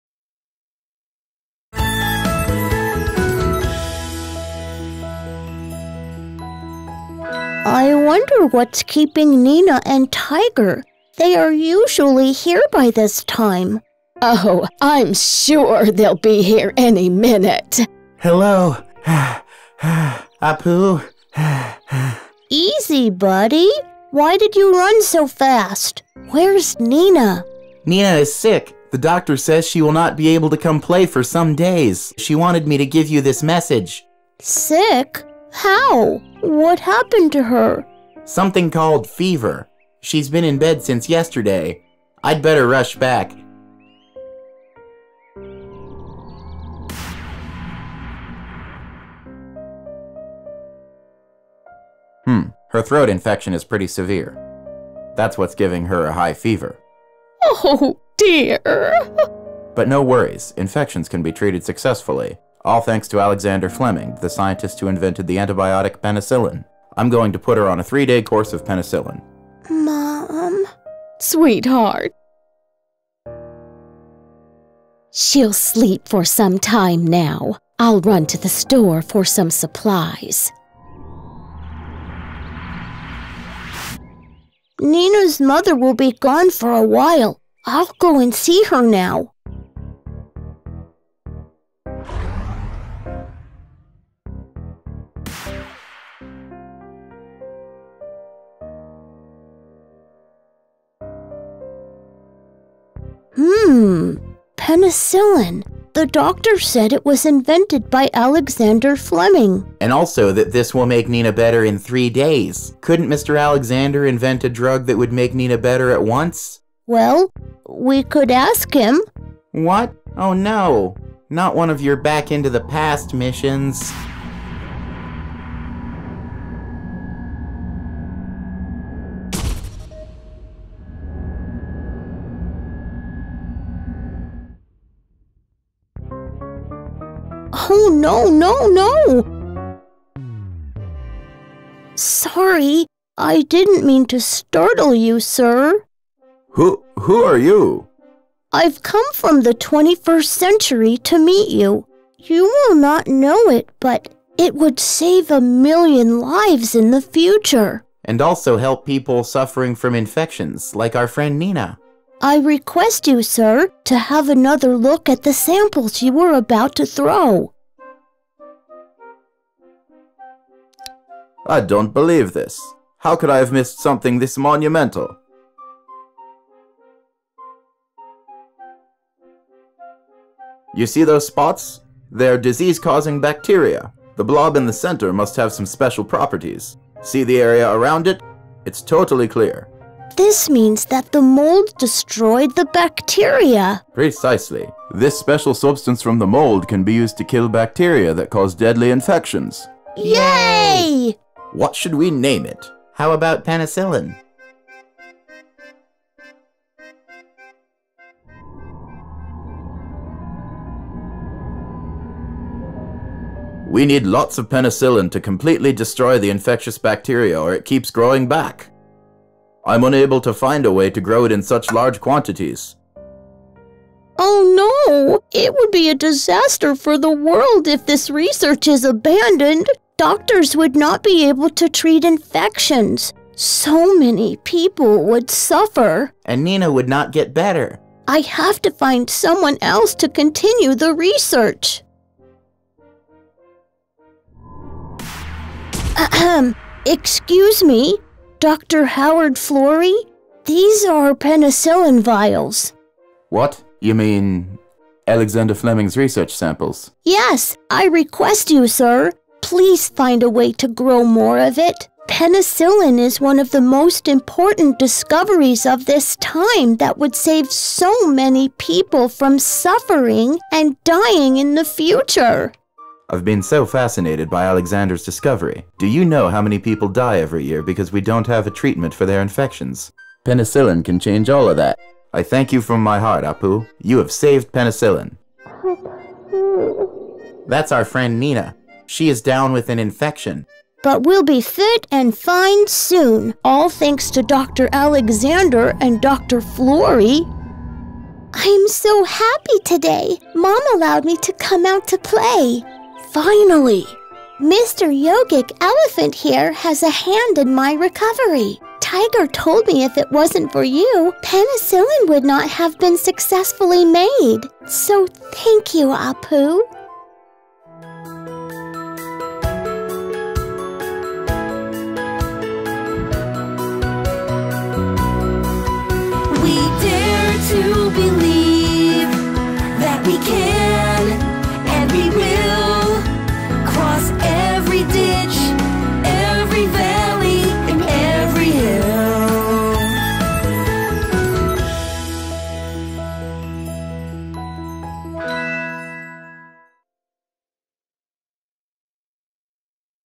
Short Stories for Kids - Appu saves Penicillin
This story brings out awareness of what is penicillin and how it can be used for the treatment of various infections. This will help your kids learn more about infections and their cure using penicillin.

Appu is anxious because it’s well past Neena’s usual visiting time and she still isn’t here! Just then he spots Tiger running towards them. Between breathless pants, Tiger tells Appu that Neena is sick with something called fever.

Terribly anxious, Appu’s Atma follows Tiger to their apartment where he hears the doctor tell her parents that she has a terrible throat infection which has given her a high fever. But it isn’t a cause for alarm as infections can be treated successfully, all thanks to Alexander Fleming, the scientist who invented the antibiotic, Penicillin.

Her parents soon leave her to rest and Appu sneaks in. He finds Neena’s forehead hot with a high fever. He remembers the Doctor saying that Penicillin would heal Neena in just a few days. Tiger wishes the scientist would’ve invented something that could’ve healed Neena faster.

Well, he just had to say it! Appu decides to see if Dr. Alexander could invent a faster-healing drug and transports them back in time, right into the scientist’s laboratory. They find him by the trash bin, about to throw a handful of Petri dishes. Appu’s super senses tell him that they are of great importance and he stops the scientist just in time! The inventor is startled out of his wits to have a purple Elephant and a dog in his laboratory but takes on Appu’s advice to have another look at samples. What he sees fills him with excitement! He finds a clear area around the mold in the Petri dishes, indicating that the mold has properties that would have destroyed the bacteria!

All Mr. Alexander needs to do now is grow the mold in numbers large enough to produce a healing drug. But the mold growing process proves to be harder than expected and the inventor abandons the research in the middle!

‘Never-say-never’ – that’s Appu’s motto! He takes the samples to another scientist, Dr. Howard Florey, who is all too excited to carry on the drug research. Their efforts pay off and result in the invention of Penicillin – the antibiotic responsible for curing infections and saving countless lives!
Neena is back to health with the Penicillin treatment.